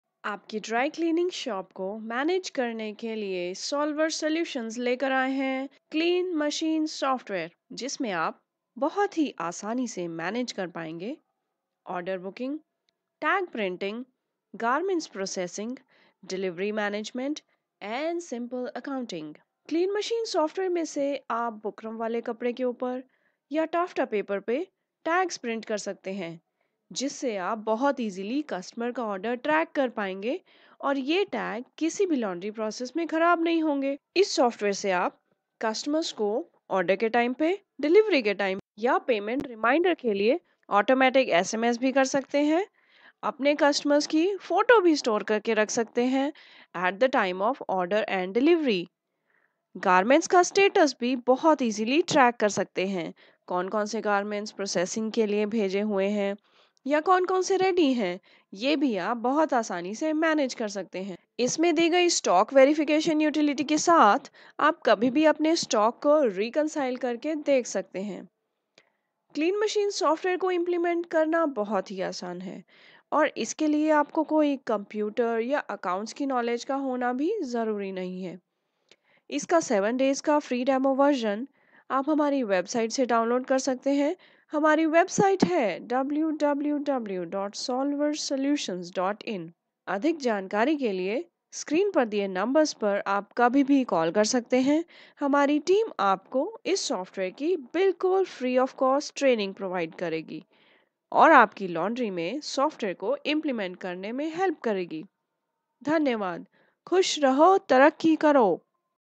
Clean Machine : Software For Drycleaners / Laundry Services By
CLEAN MACHINE: DRY CLEANERS ERP SOFTWARE

Clean Machine Software has been especially designed for dry-cleaners / laundries with automatic tag printing, Store Pictures of customer as well as items, Easy Order and Delivery Management, Complete CRM, Processing In Store, Simple Accounting and easy to operate environment.

Salient Features

·       Order Booking
·       Delivery Management
·       Tag Printing on Buckram Using Ink Jet Printer
·       Track orders in processing and make it ready for delivery in a single click.
·       Simple Accounting
·       Send Auto SMS to customer at Booking Time, Processing Time, and Items Ready for Delivery & Thanks Message at delivery time.
·       Setup your own rate list.
·       Take Photo of customer at Booking Time as well as at delivery Time.
·       Track Store Items and ready for delivery items.

To Use this software you need no training on computers, no accounting background and even only Basic English is sufficient.

As such winter, festival and marriage season has started and dry cleaners are overloaded with their working, they are looking for something that may manage their workload and reduce their Burdon. Now it’s time to display this project to some drycleaners in your market, convince them about the technical points of this software.

Print Tags on Buckram Cloth using Inkjet Printer

In Dry Cleaners Trade, A dry cleaner has a very complicated problem to manage and record the identity of particular item of a client. Many times garments are exchanged between the clients. This situation creates a big problem for a dry cleaner. It creates a question mark on his work style and goodwill.  When this project was started, we‘re also surprised over the boom of software requirements in India, We never imagined that this industry will use software for the coming 5 years. But we proved wrong.

To add on to our surprise, when we completed and displayed this project in Dry Cleaner’s association meeting in a city, we got more than 18 orders of installation.

As per our survey, this software is in demand not only in India, but also in US,  African,  and European countries.

To overcome the identification problem of each item, we covered it with ‘Tag’. The tag is 2”x2” in size and contains information such as date of order receipt, Date and time of delivery, Customer Name, Contact No, Total No. of garments etc. But on completing this project, when we installed the software on client site, and printed the tag on paper, the tag was fine. But when the garment runs through cleaning, drying and finishing process, the paper tag lost its identity. To print the tags, a special kind of paper was required which may not lose its identity when run through washing, cleaning, chemicals, drying or finishing process.  And moreover the cost of tags and printing must not increase. At this time a new research come into light. You will also get surprised to know. We used Inkjet Printer a special kind of cloth known as “Buckram” (commonly used in shirt’s Collars and cuffs) as a paper. Now this problem is 100% solved. The buckram tag once printed through any inkjet printer, may be run through any process such as cleaning, finishing etc. do not lose its identity and remains same in the condition in which we print it.

Join us on skype = Speed.Solver